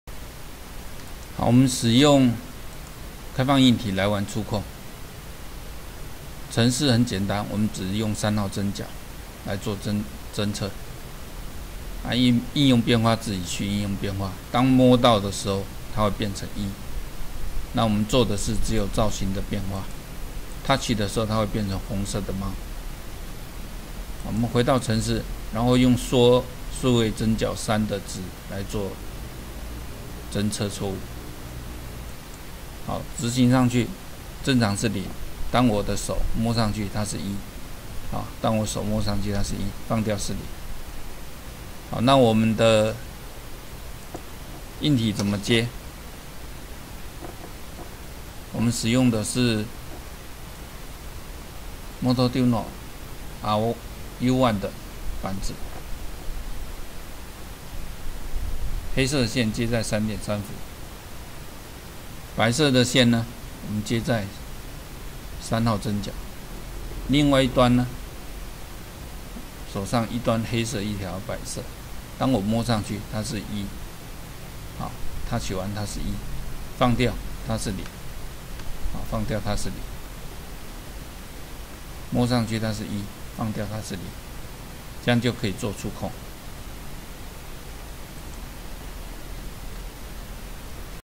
開放硬體玩觸控
使用motoduino u1做觸控測試